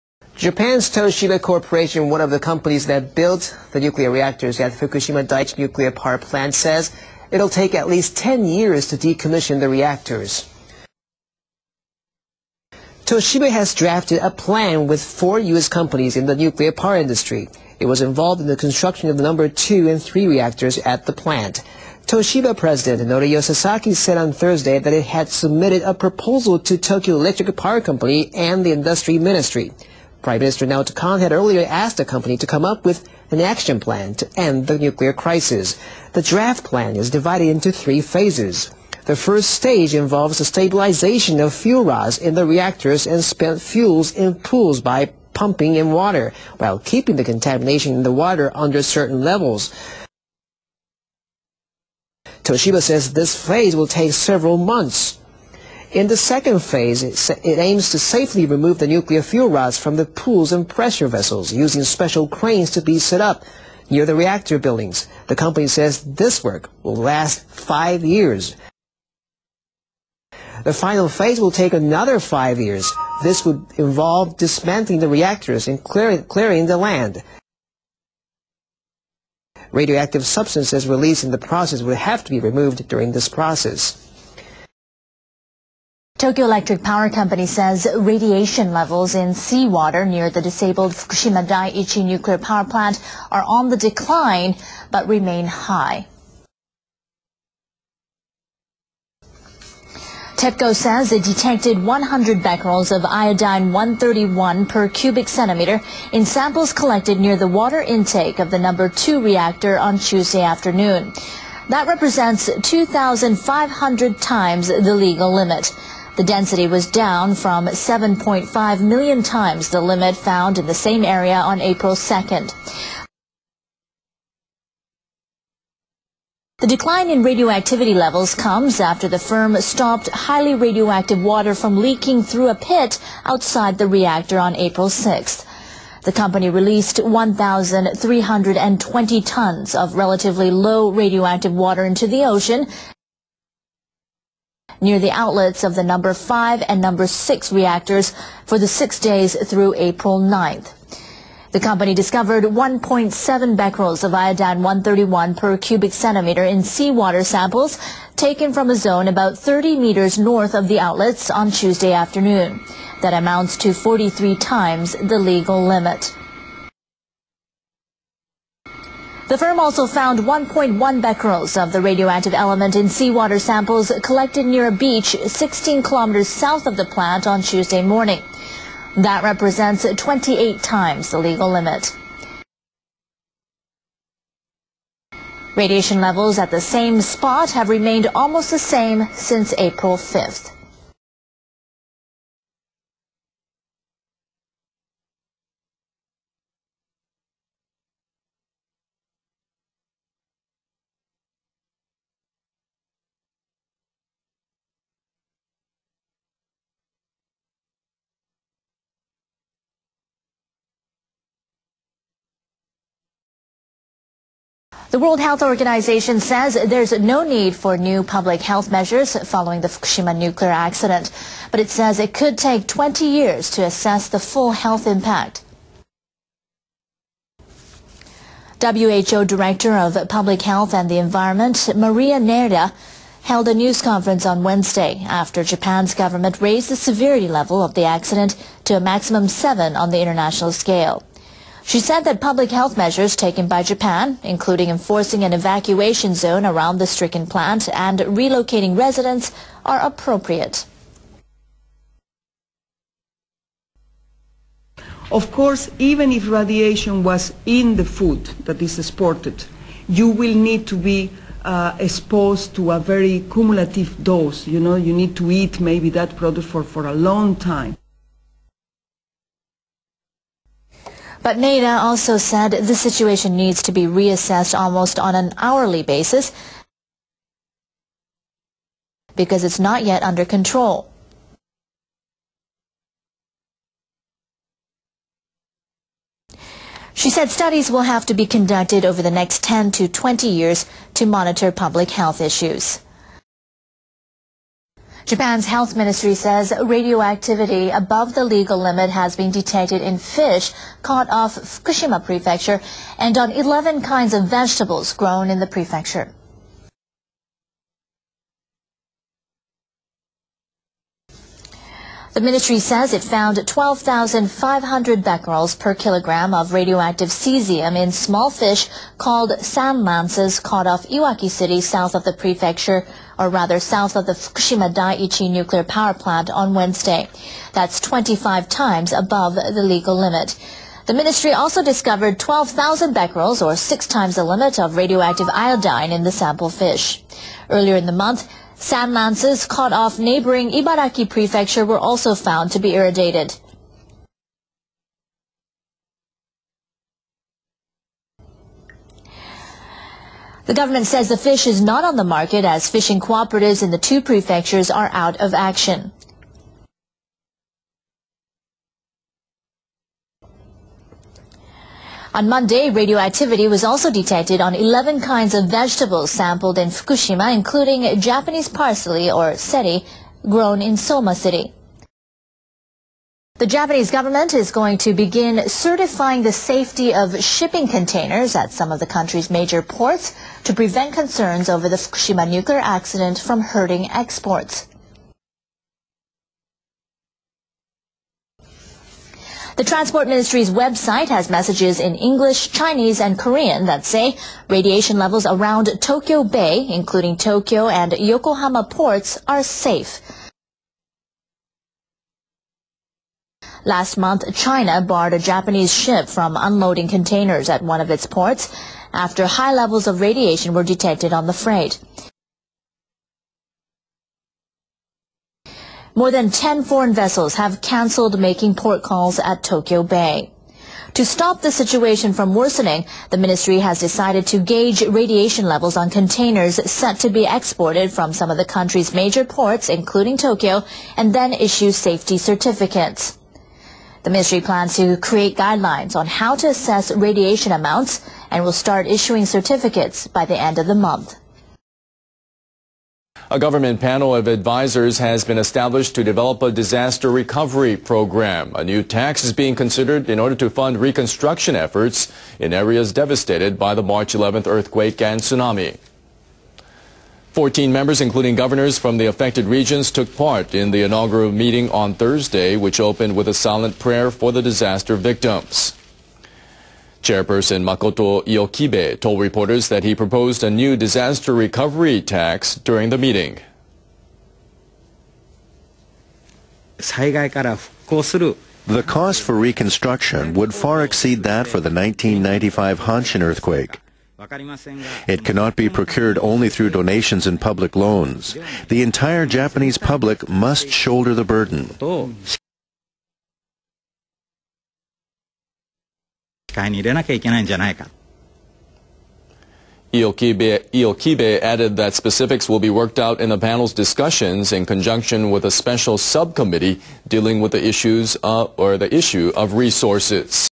Fukushima Revisited: Fukushima 10 Year Plan Announced 4/14/11
Originally published on Apr 14, 2011

WTF cat calls in back-ups.
Reactor makers draft 10-year decommission plan

Japanese manufacturer Toshiba, which helped build the Fukushima Daiichi plant's now crippled nuclear reactors, says decommissioning them will take at least 10 years.

Toshiba has drafted a plan with 4 US companies in the nuclear power industry to decommission the plant, including the Number 2 and 3 reactors that it built.

The company's President Norio Sasaki said on Thursday that it has submitted the proposal to Tokyo Electric Power and the Industry Ministry. Prime Minister Naoto Kan had earlier asked the utility to come up with a plan to end the nuclear crisis.

The proposal is divided into 3 phases. The short-term plan, likely to take several months, involves cooling and stabilizing reactors and spent fuel pools, while preventing radioactive water from increasing.
Toshiba will then move toward the medium-term plan, involving the safe removal of nuclear fuel rods from the pools and pressure vessels, using special cranes to be set up near the reactor buildings. Toshiba says this work will take 5 years.

The final phase, dismantling the reactors and clearing the land, will take another 5 years. Toshiba says that radioactive substances released in the process must be removed during this phase.

Thursday, April 14, 2011 21:41 +0900 (JST)

also, leakspinner has good conversational data about the japan fukushima situation here on youtube. just type leakspinner in the search box and you will find his channel.